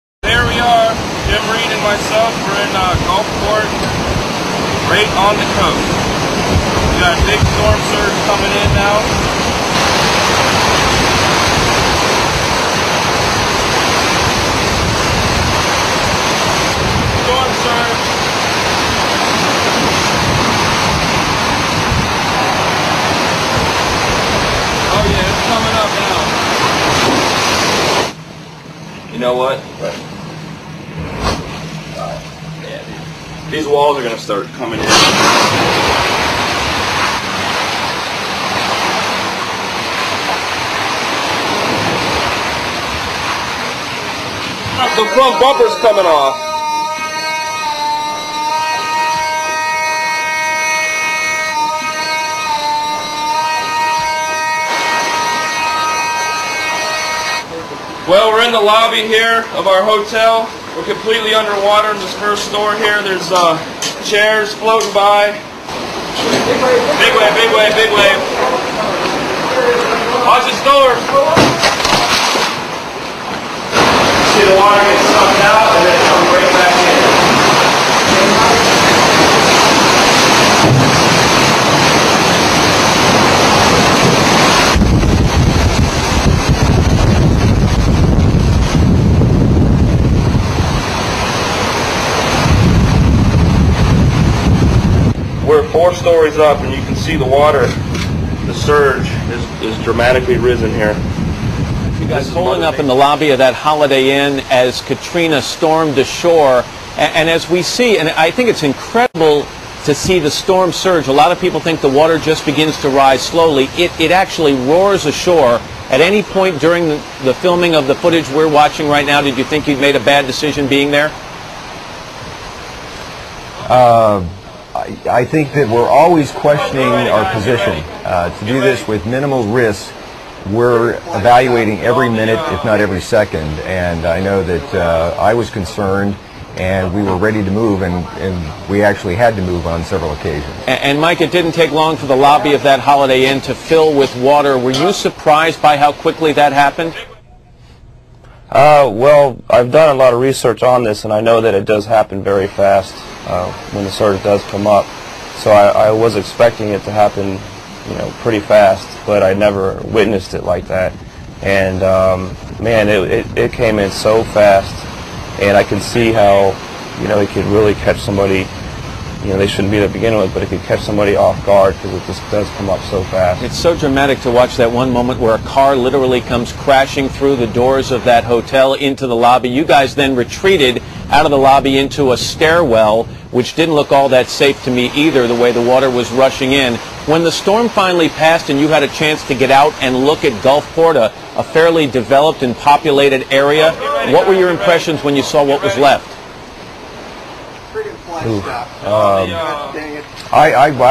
Hurricane Katrina Gulfport 2005 Extreme Climate Change
Extreme weather events like Hurricane Katrina in 2005 are known to be intensified by global warming caused climate change. Businesses and individuals must all works towards improved energy efficiency and reduced carbon emissions. Events like Hurricane Katrina can cost over $1Billion a hit and can occur several times a year. The economical and environmental costs of not acting are are far more than tacking climate change now.